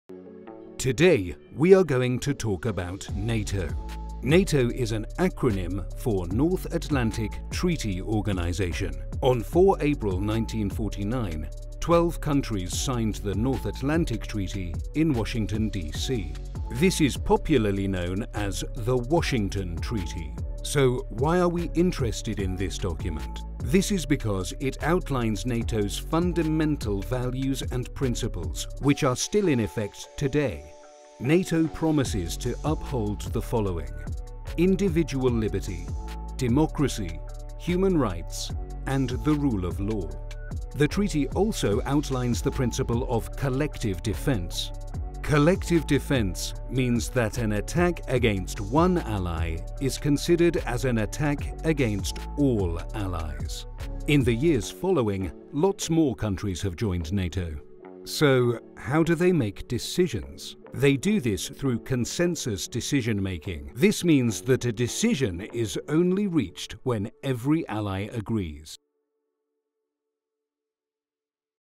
Who are NATO and what do they do?
Have you never quite understood what NATO do? Worry not, we've got you covered! In this video we explain what NATO stands for, how NATO came to be, who is a part of NATO and NATO's values.

This is part 1/4 from our miniseries on NATO.